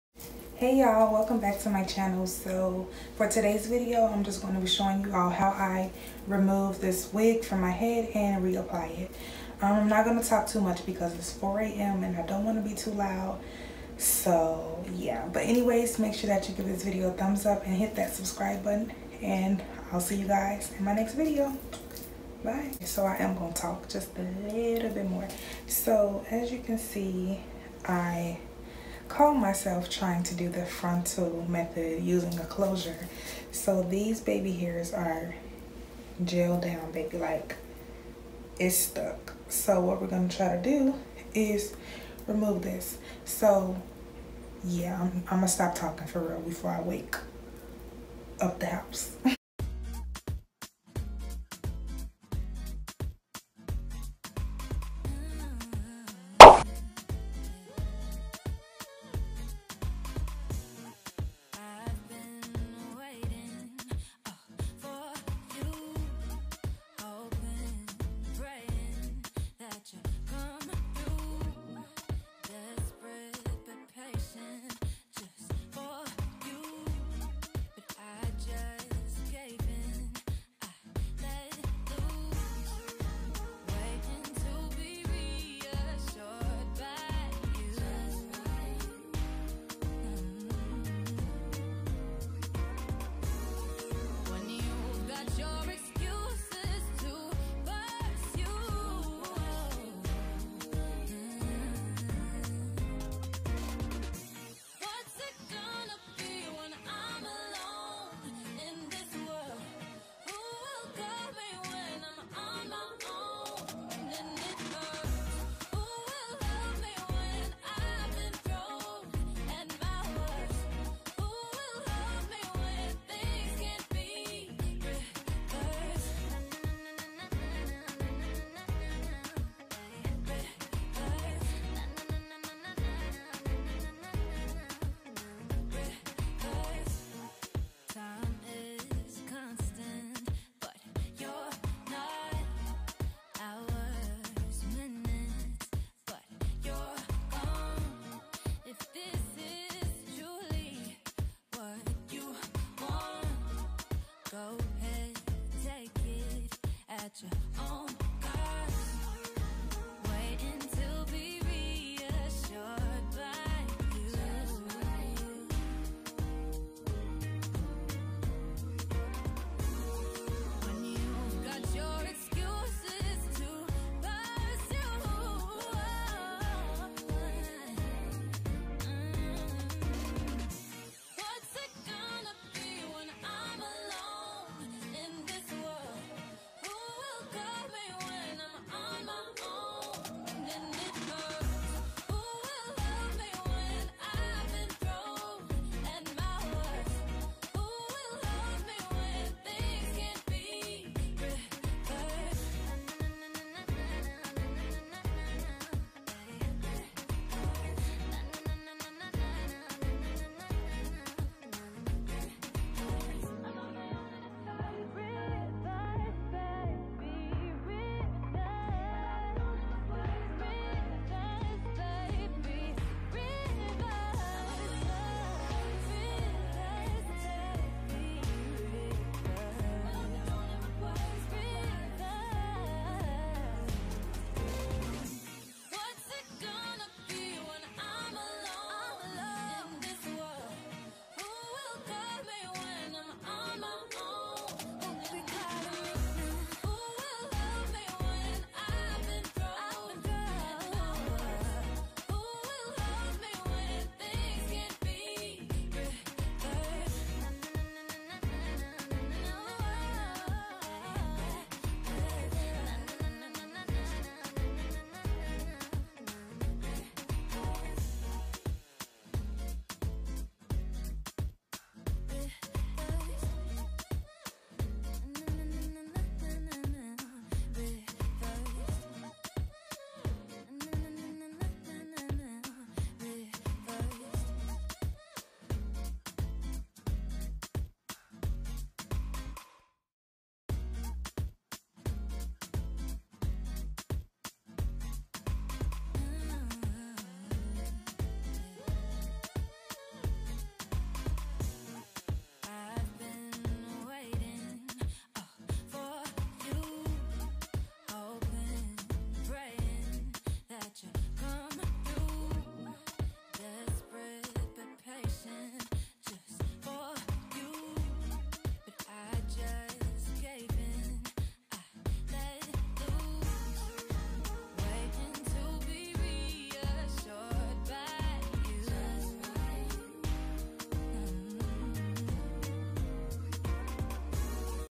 Remove Got2b from closure and reapply! || Pt.1
Watch this video to see how I remove my wigs. NEVER snatch wig off if glue/gel is applied. You can use alcohol or even oil to loosen up the gel. I decided to use peroxide because I didn’t have alcohol lol. But be very gentle! We want to save the edges. In the video you will also learn how to remove gel from your closure or frontal.

I hope you all enjoy pt.1 to this video. Part two will be uploaded shortly showing you the rest of styling this hair.

Want to collab? Have products for me to review? Need advice?  Video suggestions?

About Karin ❤️

What do I film with? iPhone 8